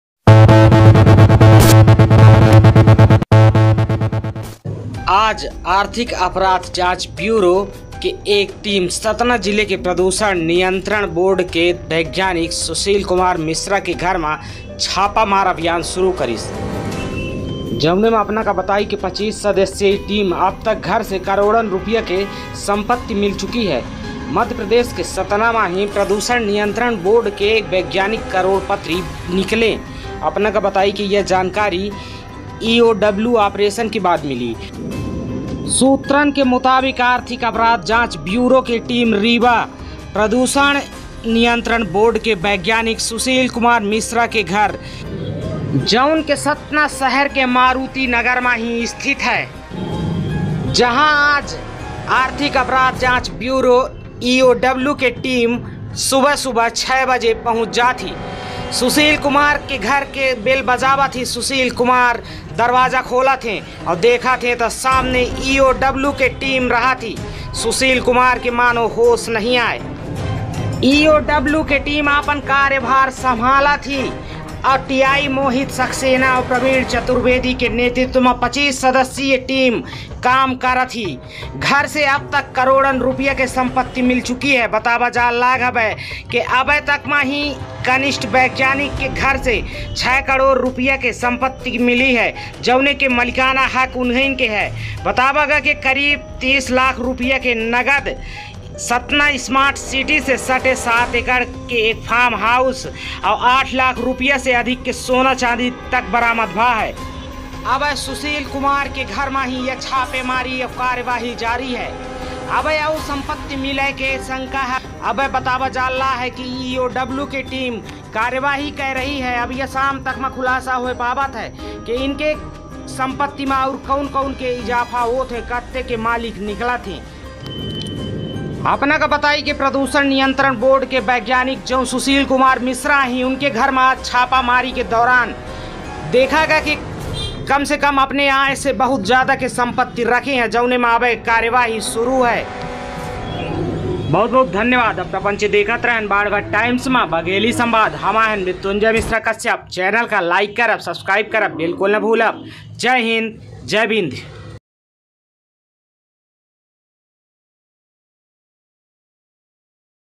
Satna || अधिकारी के घर का सुबह खुला दरवाजा| अधिकारी के उड़े होस || कारण था गरीबों का चूसा हुआ धन
आर्थिक अपराध जांच ब्यूरो की एक EOW टीम ने सतना जिले के प्रदूषण नियंत्रण बोर्ड के वैज्ञानिक सुशील कुमार मिश्रा के घर पर छापामार अभियान शुरू किया है. 25 सदस्यीय टीम को अब तक घर से करोड़ों रुपये की संपत्ति मिल चुकी है.
मध्य प्रदेश के सतना में प्रदूषण नियंत्रण बोर्ड के एक वैज्ञानिक करोड़पति बन गए हैं। यह जानकारी ईओडब्ल्यू ऑपरेशन के बाद मिली, ऑपरेशन अभी भी जारी है। आर्थिक अपराध जांच ब्यूरो की एक EOW टीम ने सतना जिले के प्रदूषण नियंत्रण बोर्ड के वैज्ञानिक सुशील कुमार मिश्रा के घर पर छापामार अभियान शुरू किया है.

सूत्रों के मुताबिक आर्थिक अपराध जांच ब्यूरो ( EOW) की रीवा टीम ने प्रदूषण नियंत्रण बोर्ड के वैज्ञानिक सुशील कुमार मिश्रा के घर पर छापेमारी की. बताया जाता है कि सुबह छह बजे EOW की टीम सतना के मारुति नगर स्थित वैज्ञानिक के घर पहुंची. दरवाजे की घंटी बजाने के बाद वैज्ञानिक खुद दरवाजा खोलने पहुंचे। जब उसने पार्टी के बारे में सुना, तो वह आहें भर कर नीचे गिर पड़ा। टीम ने कार्यभार संभाला और आगे की गतिविधियां शुरू कीं।